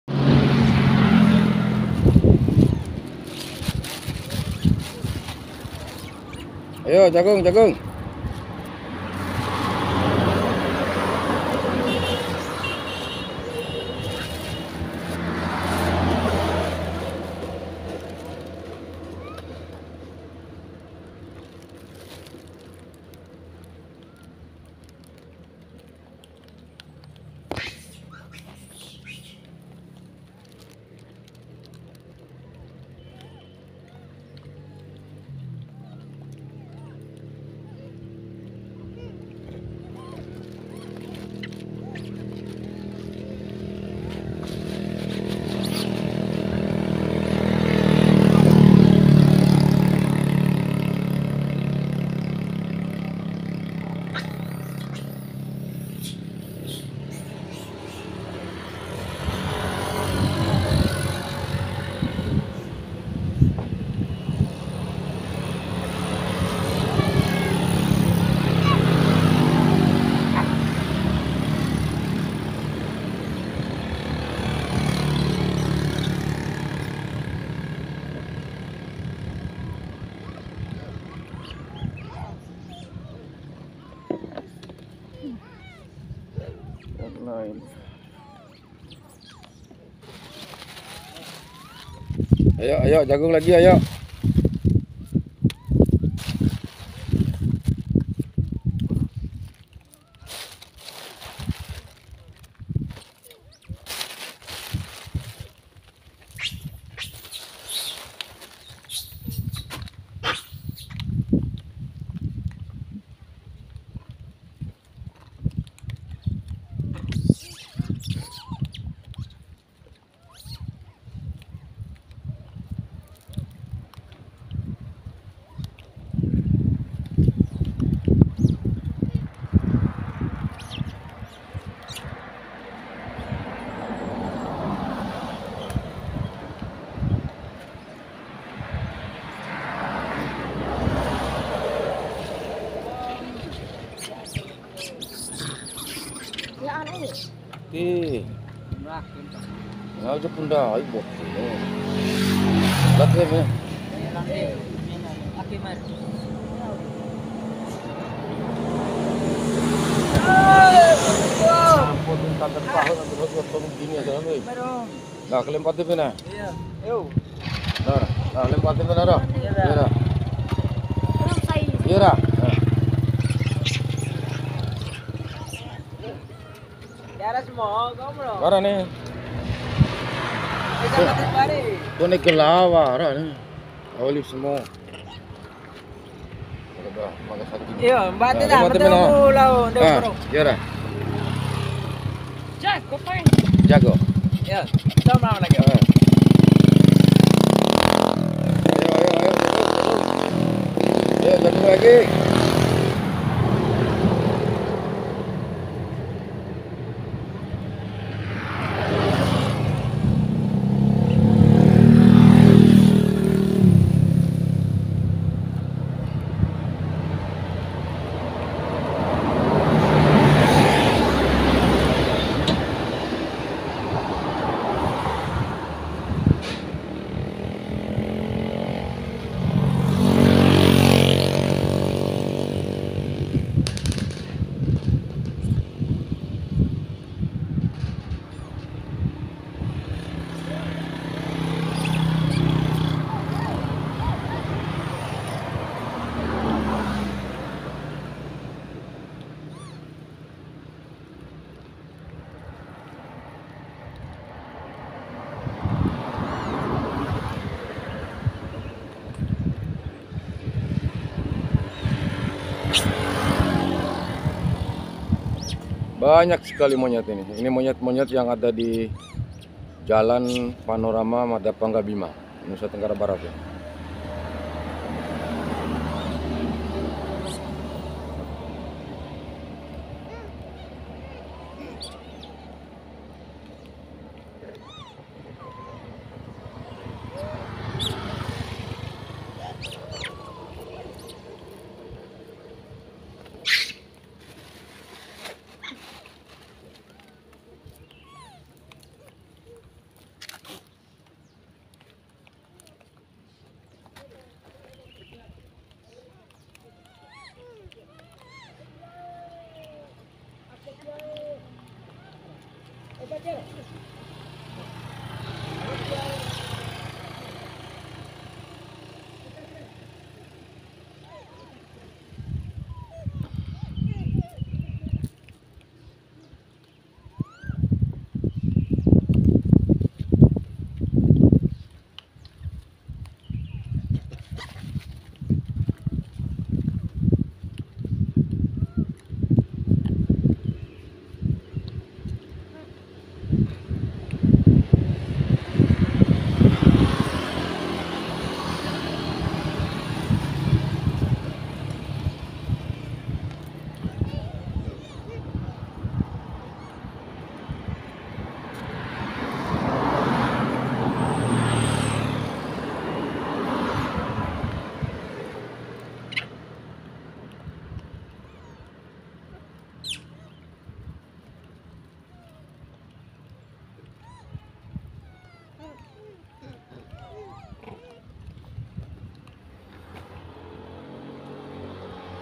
saksikan ribuan monyet serbu jalan raya
ada ribuan monyet yang sedang serbu jalan raya jalan antar kota bima dompu @NcokiMori1670